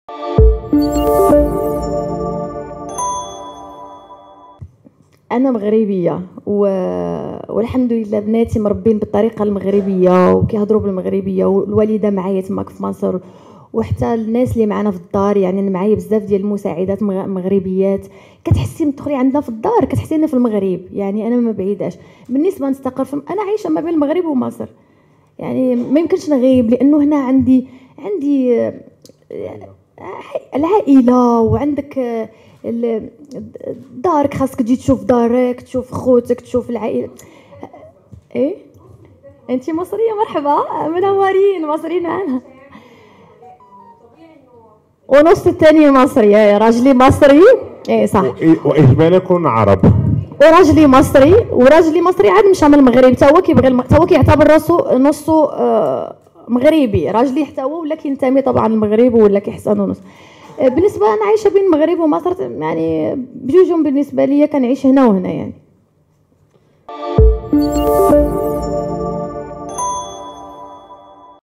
الفنانة جنات: أعيش بين المغرب ومصر وطقوسي مغربية
على هامش مهرجان موازين الذي تحتضنه مدينة الرباط، كشفت الفنانة المغربية جنات عن مكان آقامتها، واللهجة التي يتحدث بها أبناؤها.